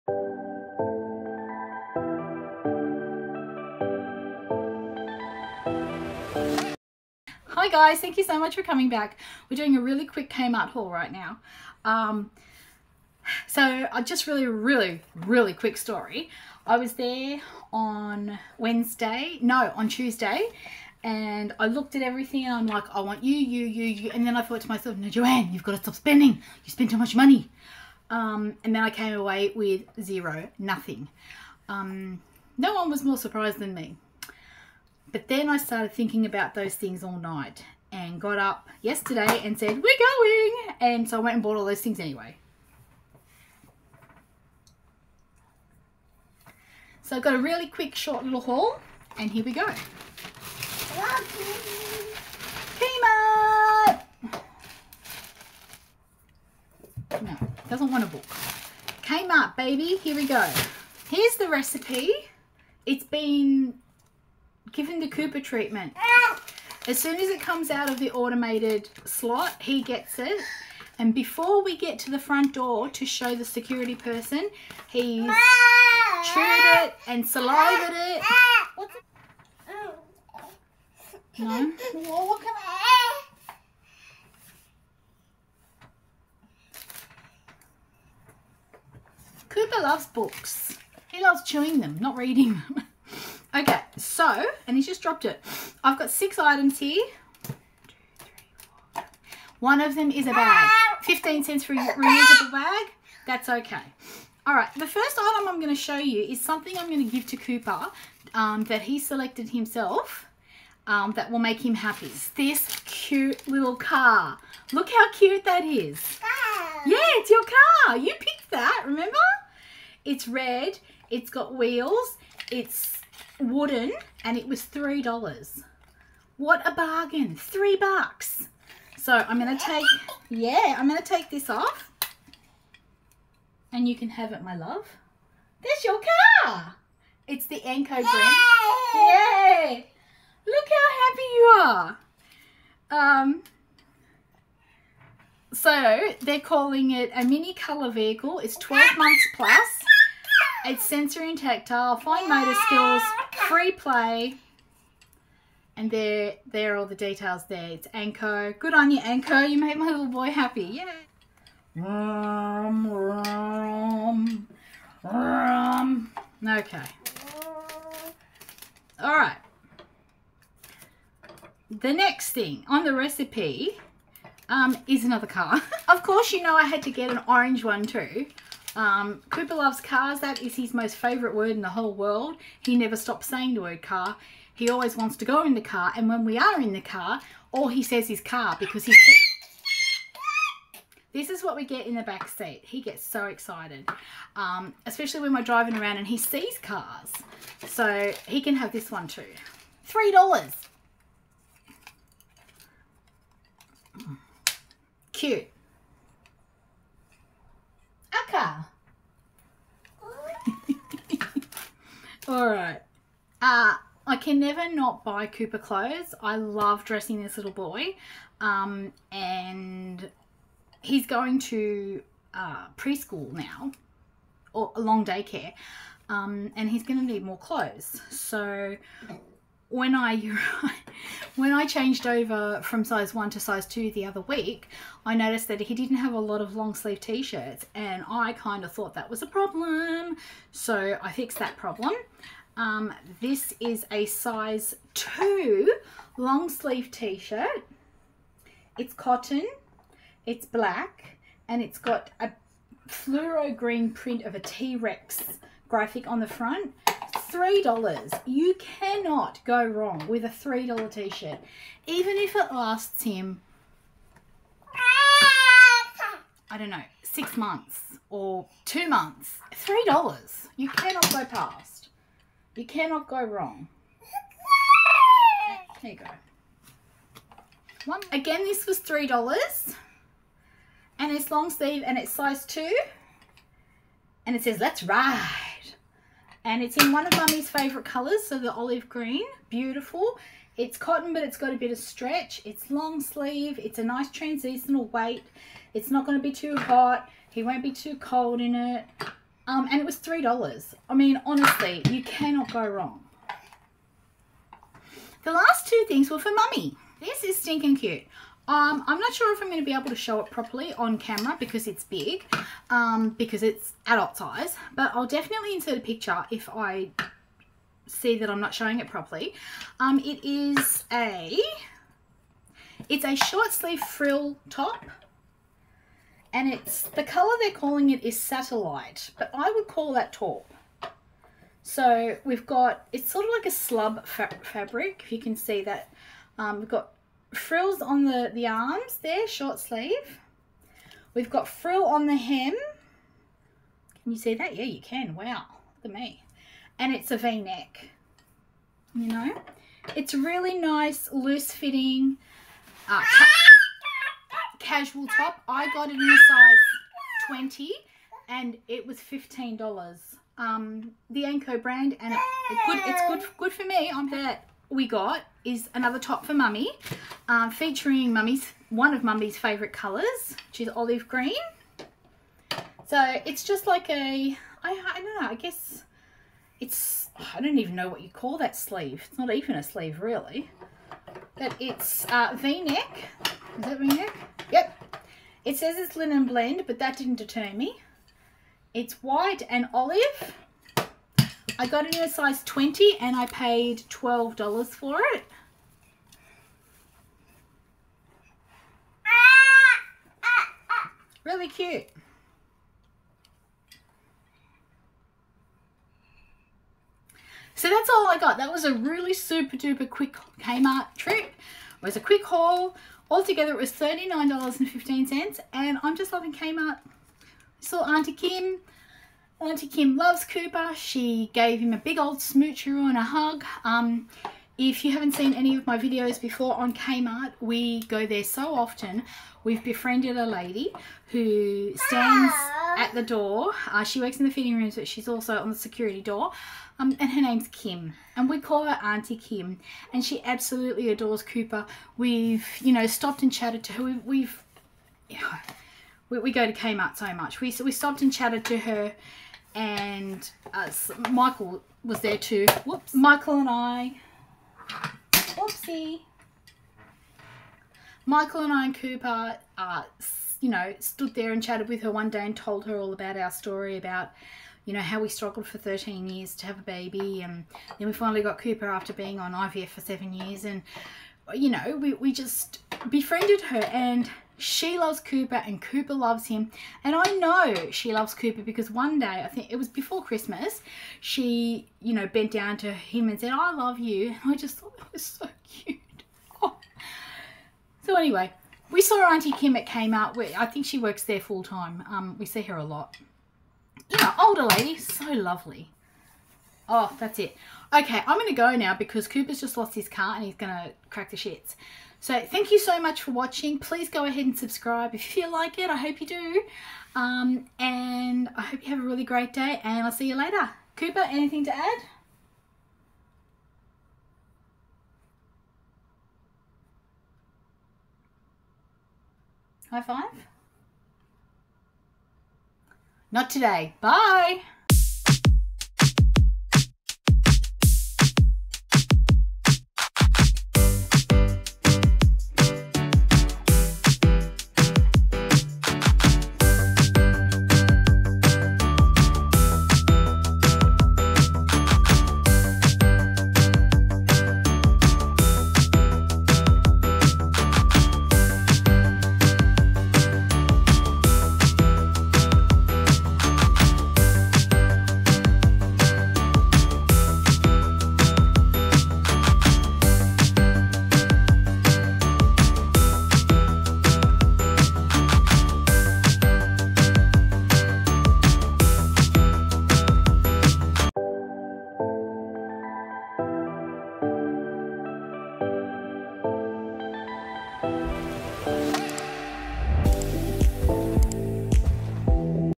❤️😍 Kmart mini HAUL!👶🚗
Cooper and I show you all the goodies we got from Kmart!